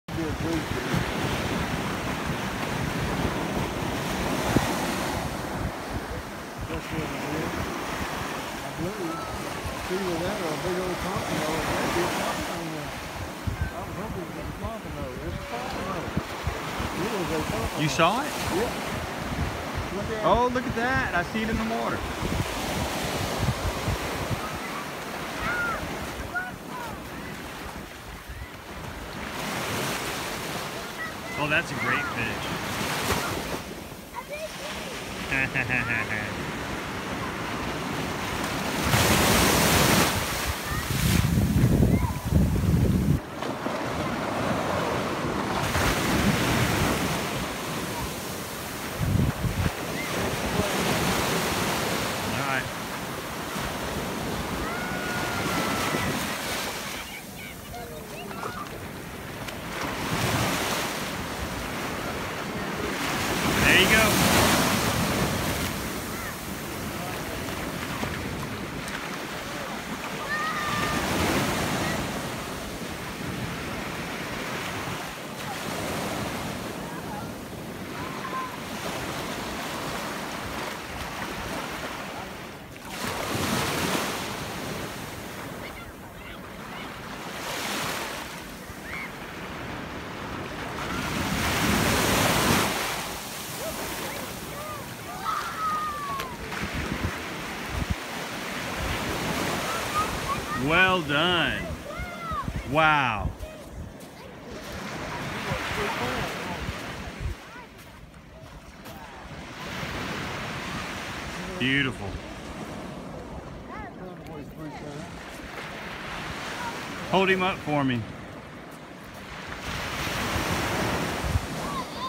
Dad‘s big pompano
This is the Pompano Bill Miller caught at Saint George Island on May 12, 2018. His son John Miller was filming.